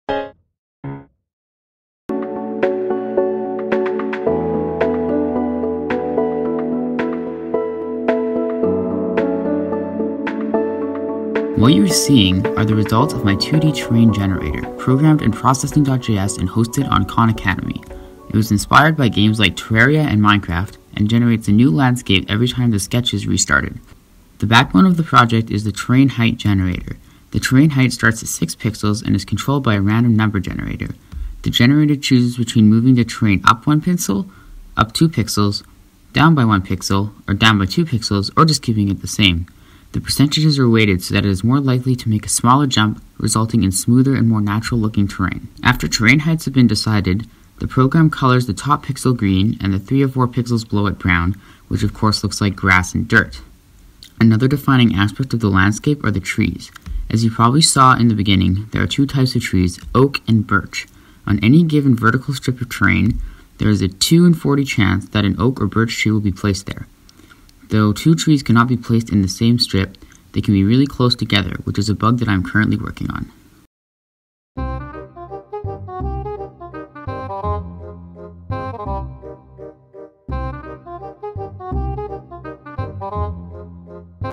Procedural 2D Terrain Generation in Processing.js
In this video, I show off my Processing.js terrain generator, and explain how some of the code works. The sketch is available on Khan Academy for anyone who wants to play around with it.